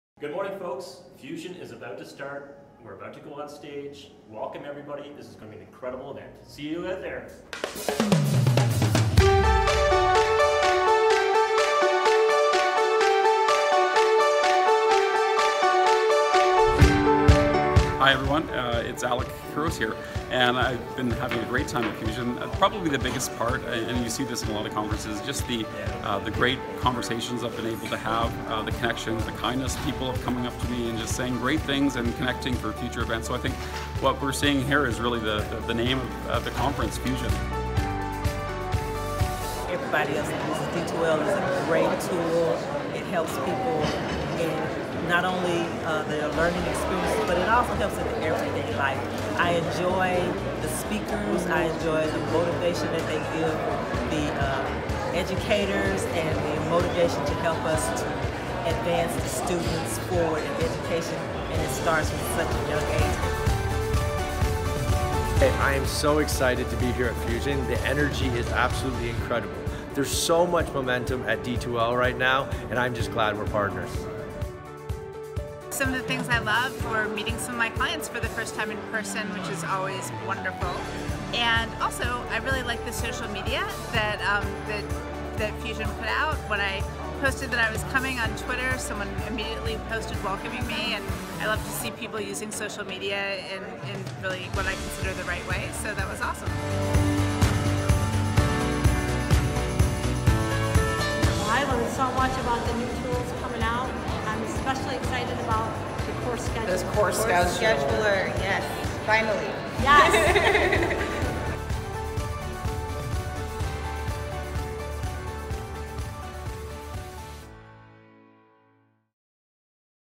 Fusion 2018
Thank you to everyone who joined us in Houston for Fusion! We had so much fun! Relive some of the best moments from the 2018 event and pre-register for Fusion 2019